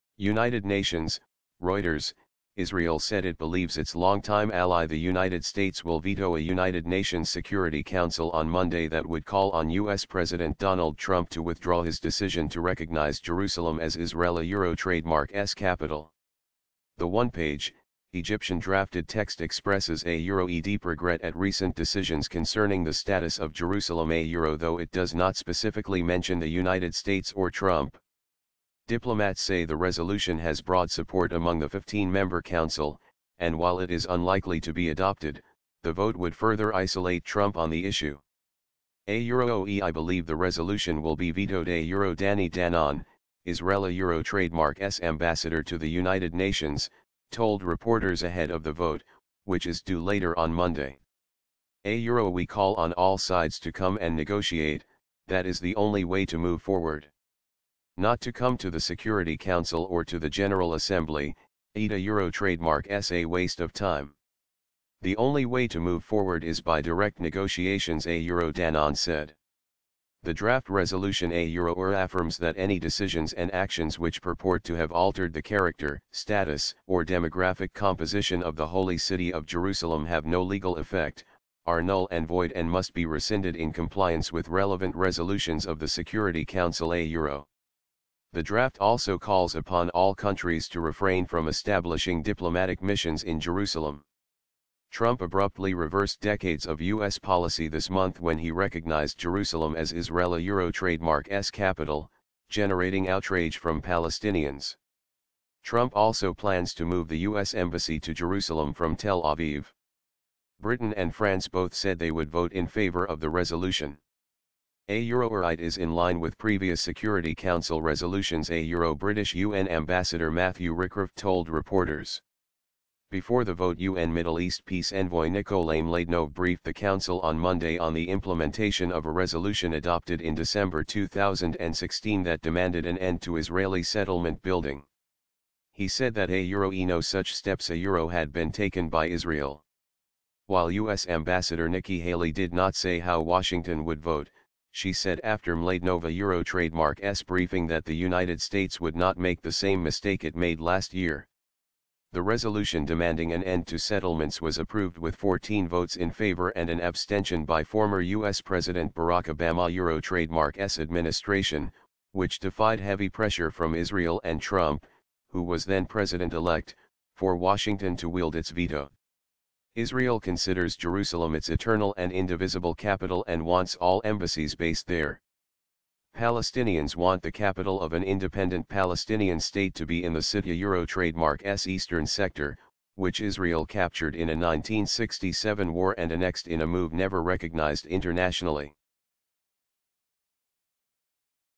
News Today 12/18/2017 | Donald Trump | Israel Believes U.S. Will Veto U.N. Resolution On Jerusalem
Israel said it believes its longtime ally the United States will veto a United Nations Security Council on Monday that would call on U.S. President Donald Trump to withdraw his decision to recognize Jerusalem as Israel's capital.